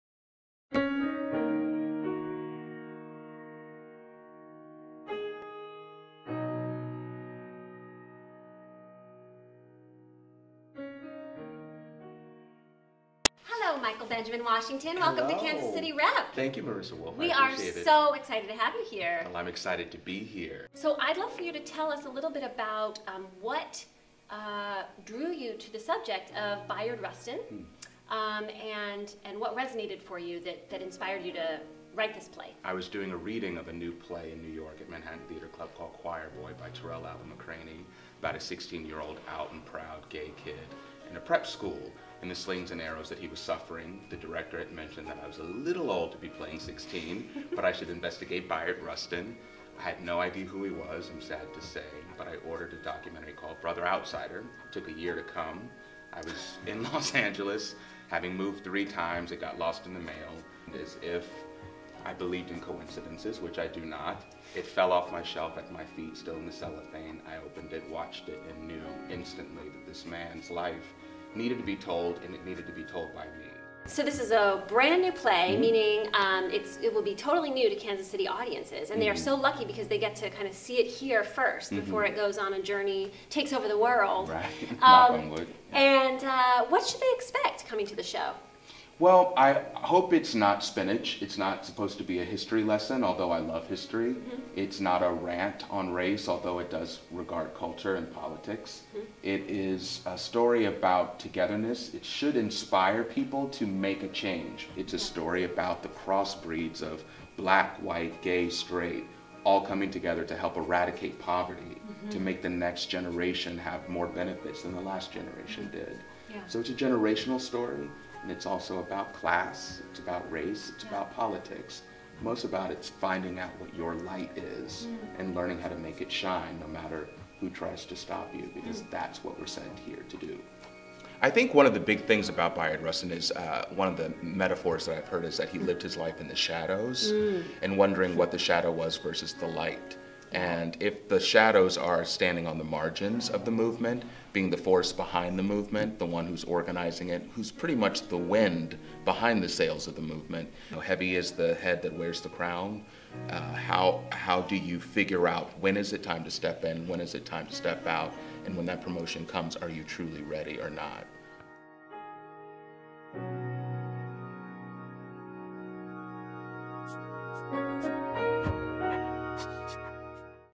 Interview With The Playwright - Michael Benjamin Washington
Learn more about Michael Benjamin Washington, writer and star of Blueprints to Freedom, in this exclusive interview with Marissa Wolf, Director of New Works at KC Rep.